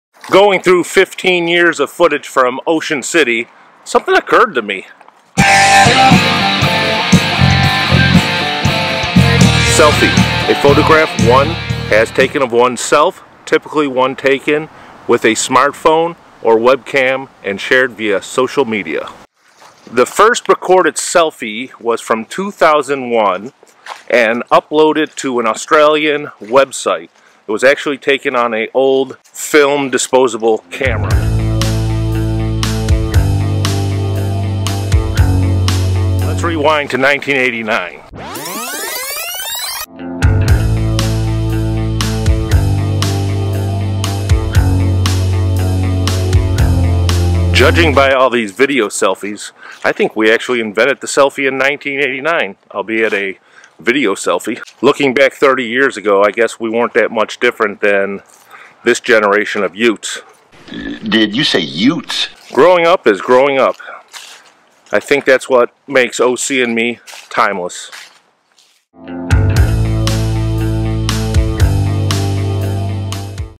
OC & Me Selfies
In 1989 we may have invented the selfie, albeit with a Sony Camcorder.